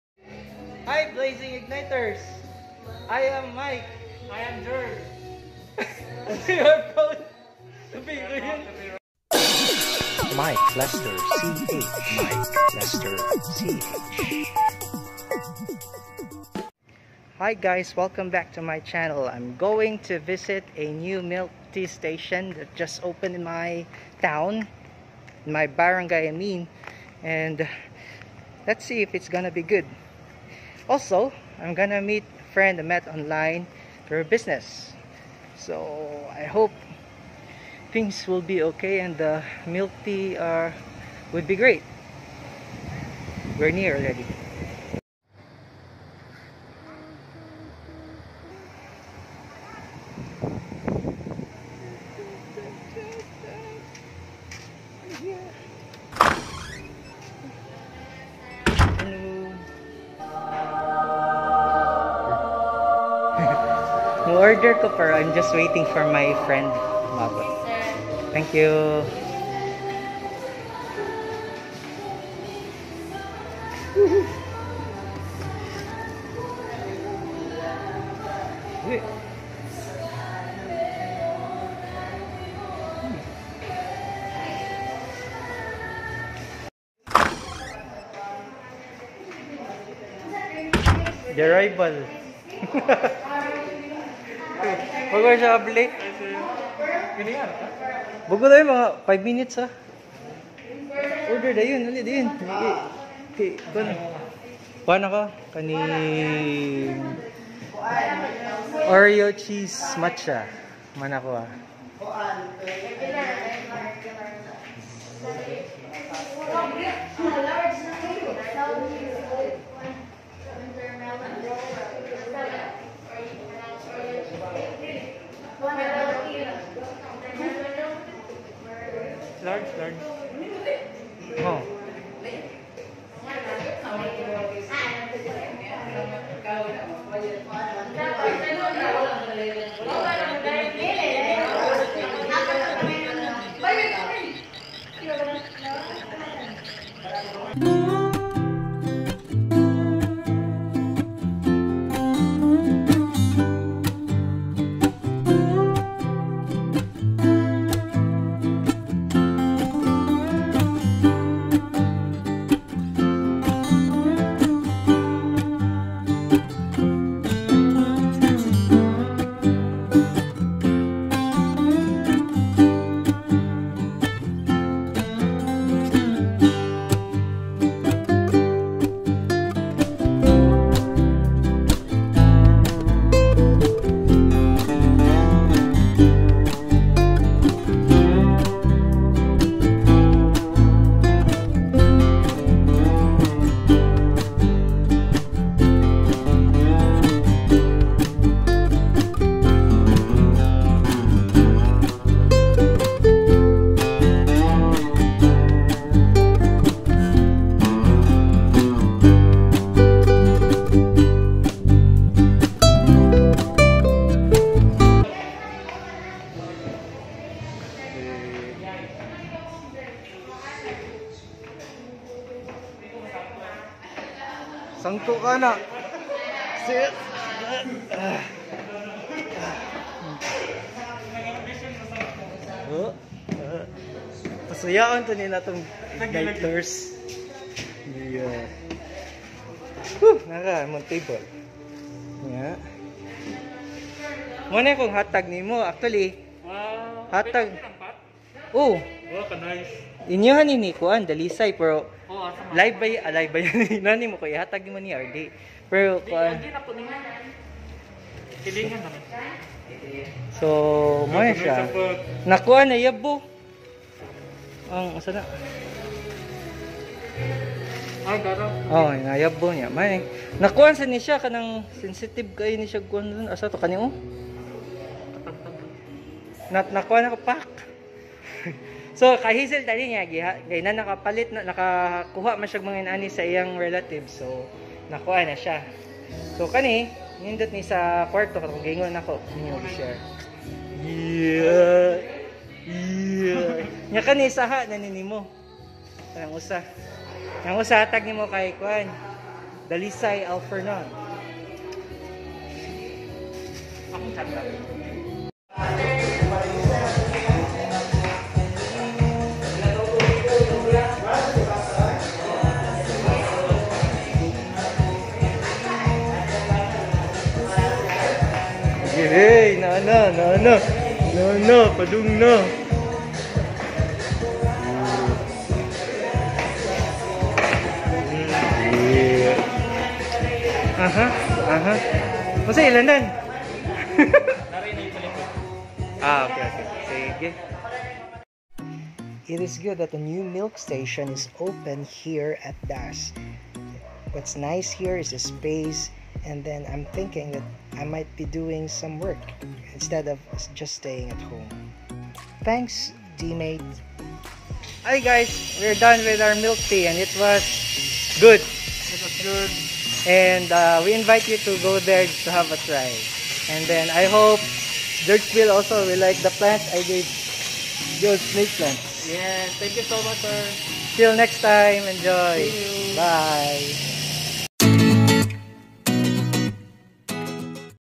Trying Teamate Milk Tea at Lutopan, Toledo City
Teamate has just opened at DAS, Toledo City. That's worth a visit. Likewise, it's an opportunity to meet an online friend around DAS (join us next time Dalisay and Fritzie) and give some of my Sansevieria hahnii.